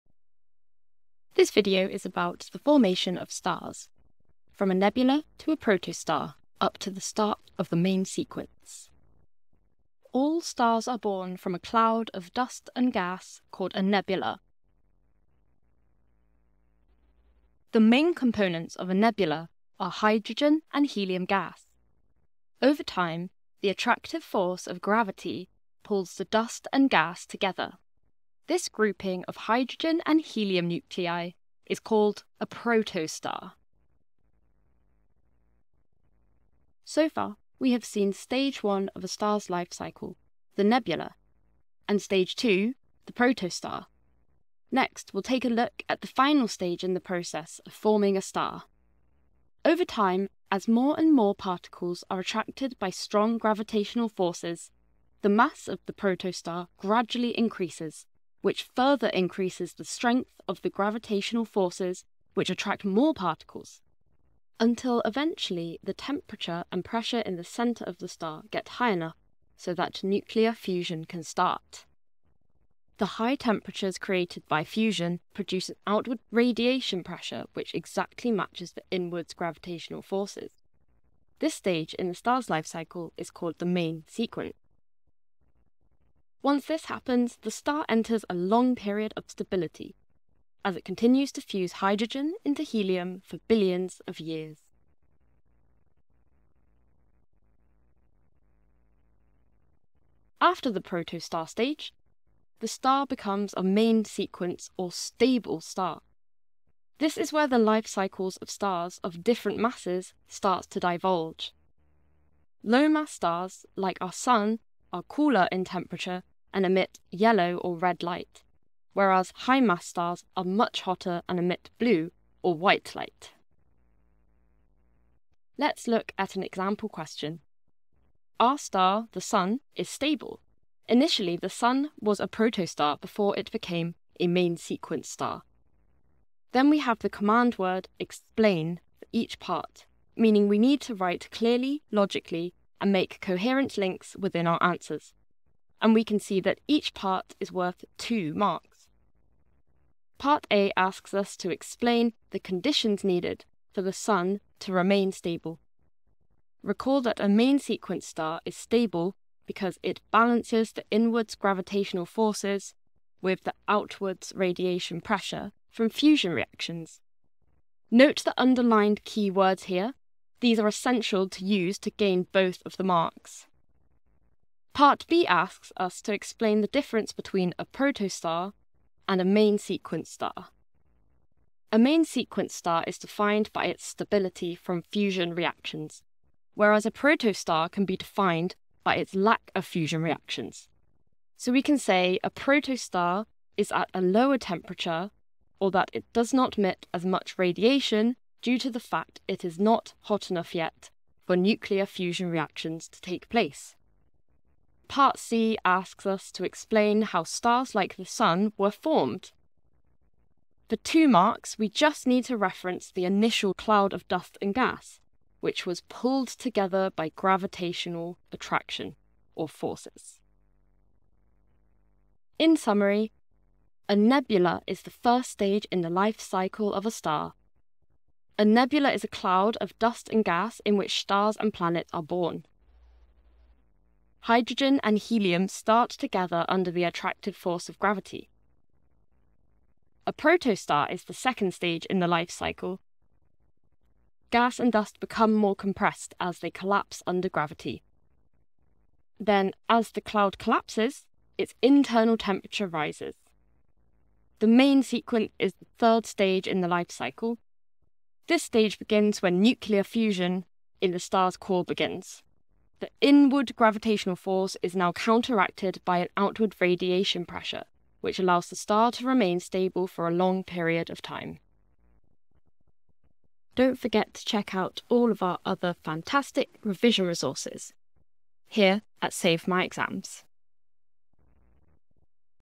6.2.2 The Formation of Stars - GCSE & IGCSE Physics - relevant to ALL exam boards
Katie explains all you need to know about the formation of stars for your GCSE and IGCSE Physics exams., from a nebular to protostar up to the start of the main sequence

What You'll Learn:
✅ Stage 1: What a Nebula is
✅Stage 2: What a Protostar is
✅Stage 3: What a Main Sequence star is
✅A worked example of a question that could come up in your exam

Chapters:
00:10 Star Formation - how are stars formed
00:19 Nebula to Protostar
00:49 Protostar to Main Sequence star
01:40 Main Sequence Stars
02:05 Worked Example
03:44 Summary